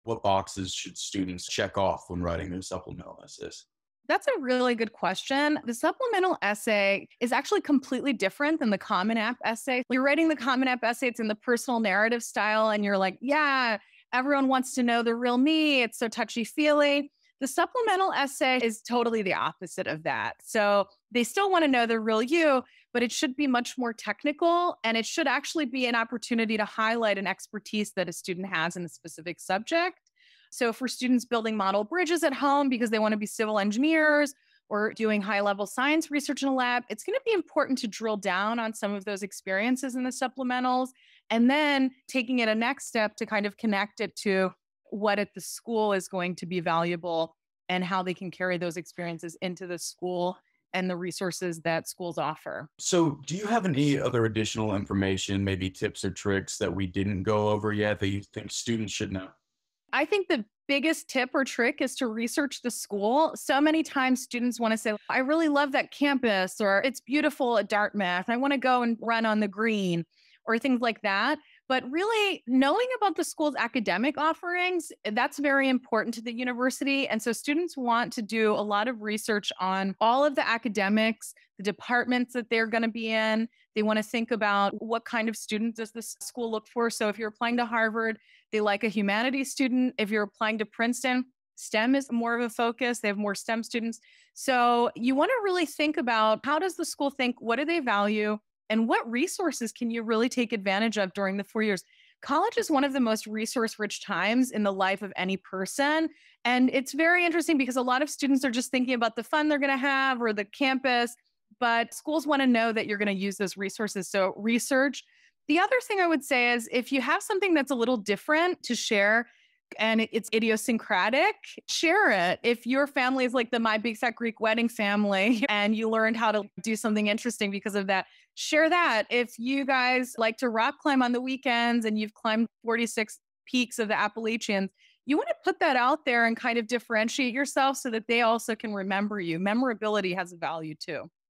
Tips for Writing Supplemental Essays
Interview with Sylvia Vukosavljevic, a college admissions consultant with Veritas Prep